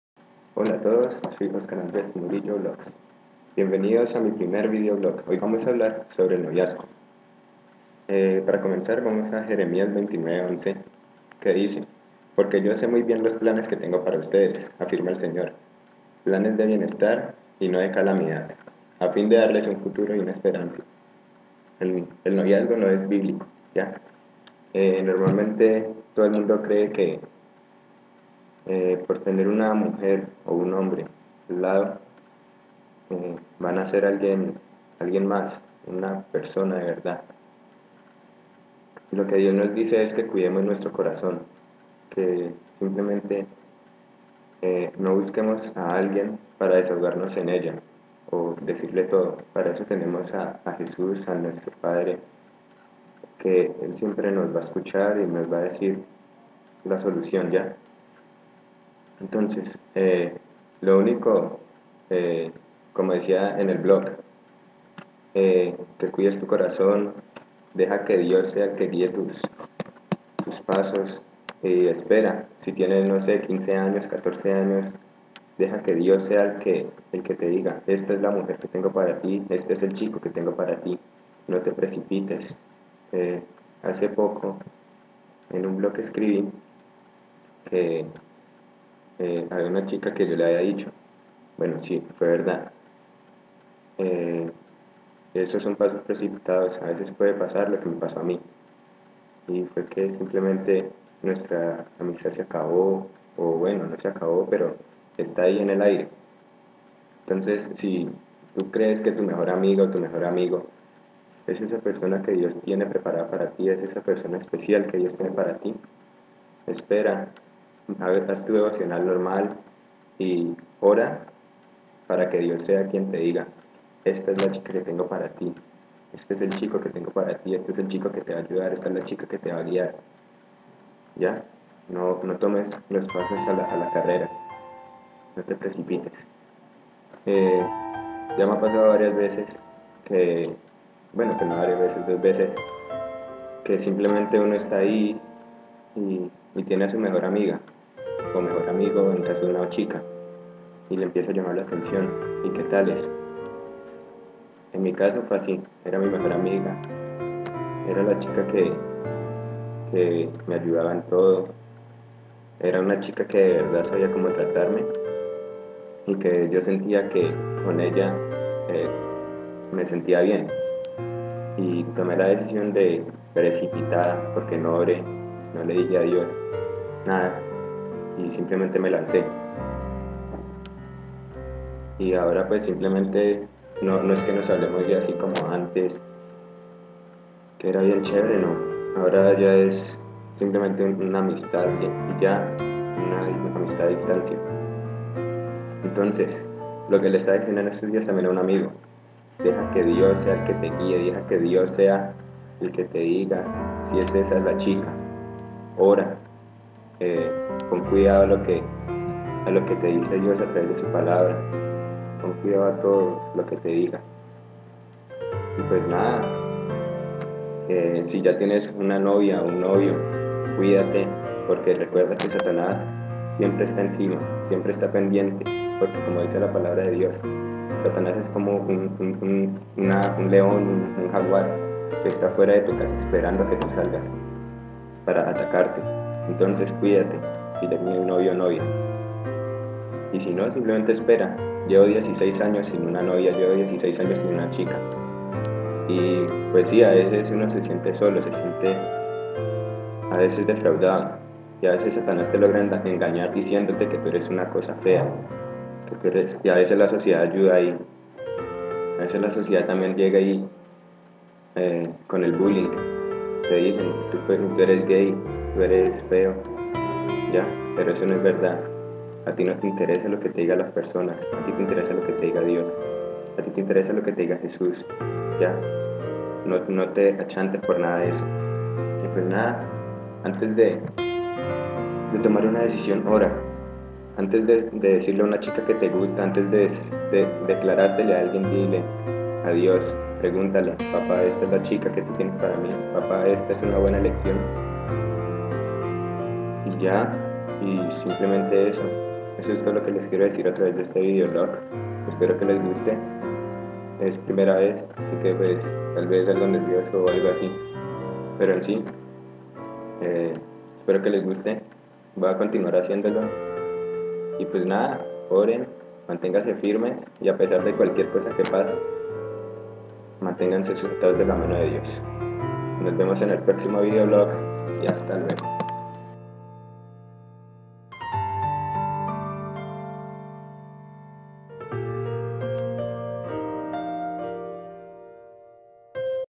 El Noviazgo
Este vídeo esta dedicado a cada uno de esos jovenes que estan desesperados por hallar novi@.